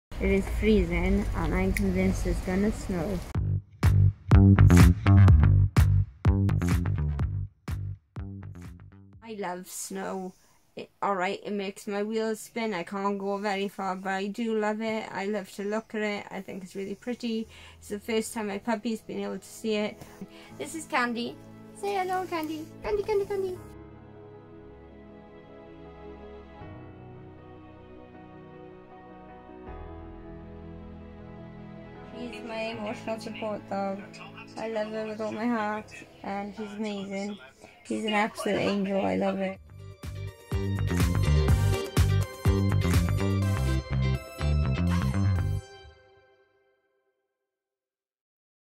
unlockedlives: Sasha's video #4
The unlockedlives video project explores the narratives of Self in the present moment from the standpoint of a diverse group of disabled people. It investigates and portrays the attitudes of a core ensemble, enquires about their hopes and fears, and captures instances of home life as narrated in first person and without filters, agendas or intermediaries.

The setting of a safe on-line workspace has given our participants a framework to offer a record of their life and thoughts during the protracted Covid19 social and medical crisis.

Accessible videomaking resources, and an emphasis on ‘demystifying the tech’ while encouraging a commitment to output, has fostered enjoyable skill-building, and has helped creating a great deal of compelling and powerful moments of self.

The project has grown through the challenges posed by the ever-changing Welsh Coronavirus landscape, and what was initially thought of as a celebration of regained freedom in a post-Covid Wales, has instead focused on capturing the private, everyday issues faced by our members through the waves of this pandemic, and contributed to the creation of a repository of self-expressive content.

This is a montage of some of the content created during the project.